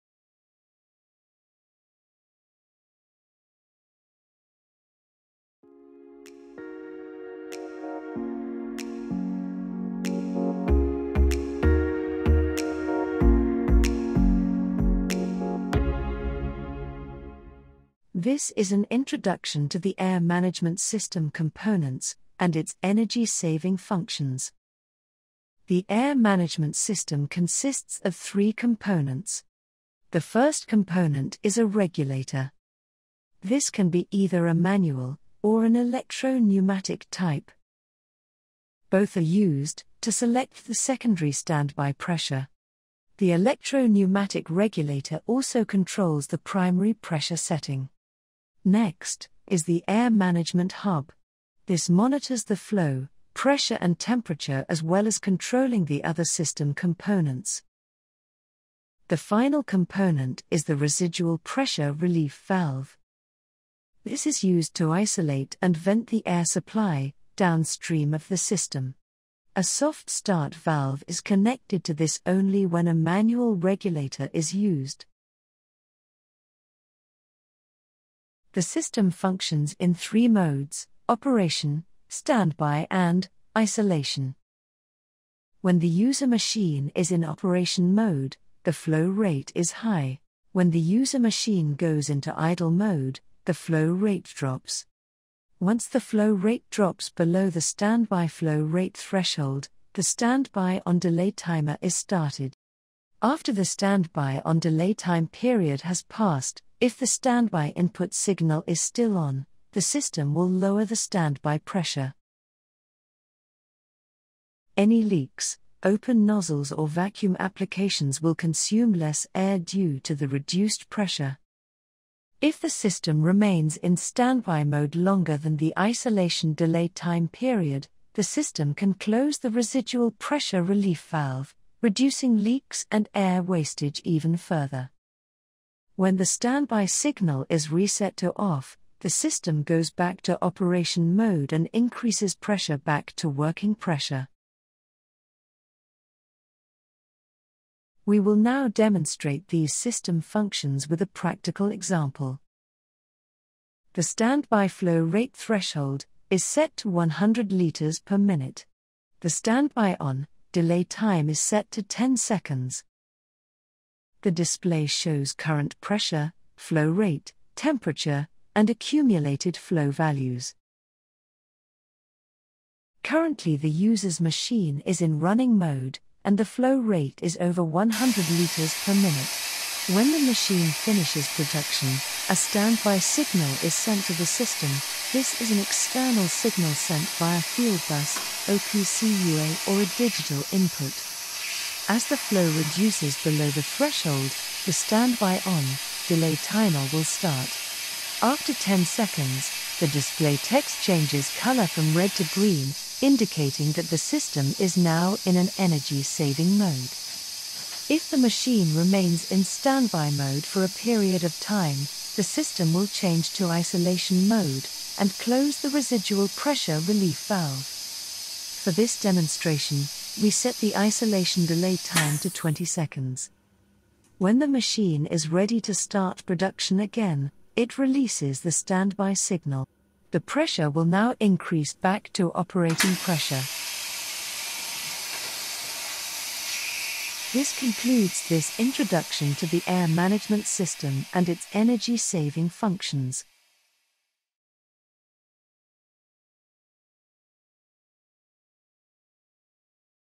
[SMC 메인터넌스] 에어 관리 시스템 AMS20/30/40/60 Series 작동 원리
SMC 에어 관리 시스템 AMS20/30/40/60 시리즈의 작동 원리에 대한 영상입니다.
SMC AMS20/30/40/60 시리즈는 공장 전력 소비 최적화를 위한 최적의 솔루션 제품으로 공장 에너지 절약과 탄소 절감에 기여합니다.

00:00 – 소개
00:27 – 주요 구성 요소
01:23 – 주요 기능: 작동 모드
01:44 – 주요 기능: 대기 모드
02:08 – 주요 기능: 차단 모드
02:32 – 시연